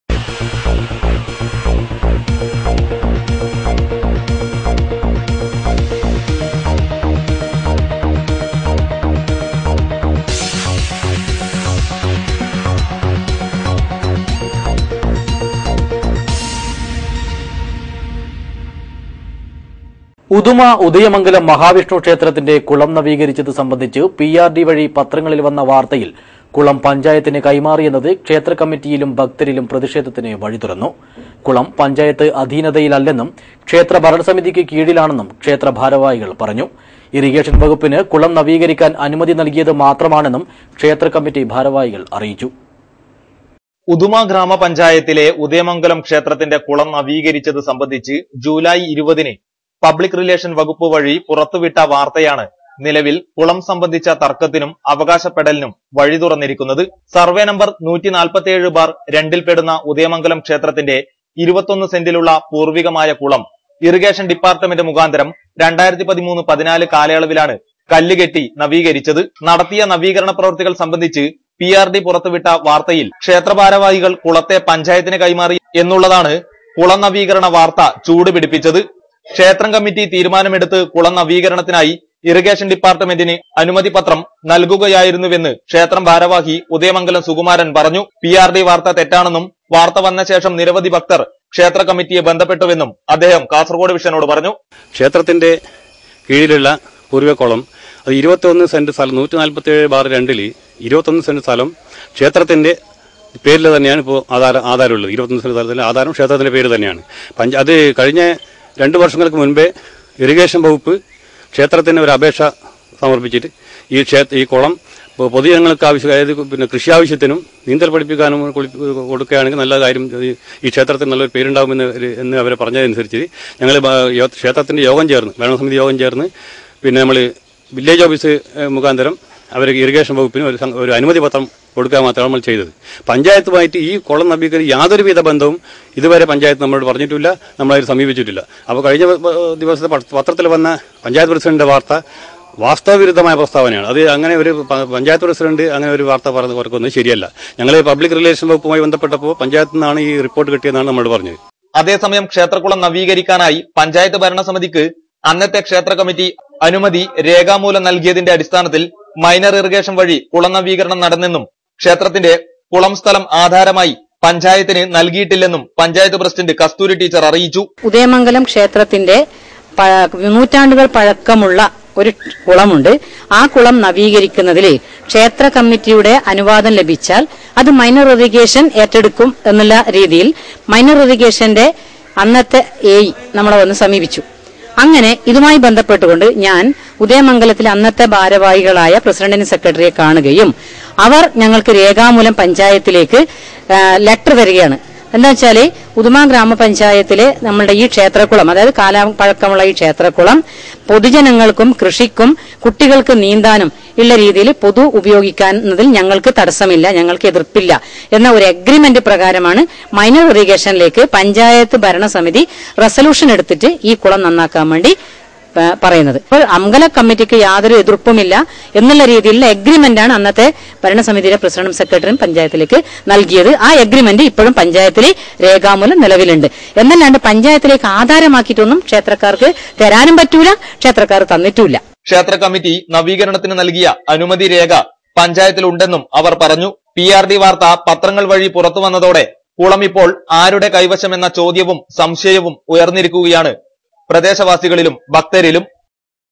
KASARAGOD VISION NEWS udayamangalam kulam ഉദയമംഗലം
ഉദയമംഗലംക്ഷേത്രക്കുളംനവീകരണംഭാരവാഹികളുംപഞ്ചായത്തുംഇരുതട്ടില്‍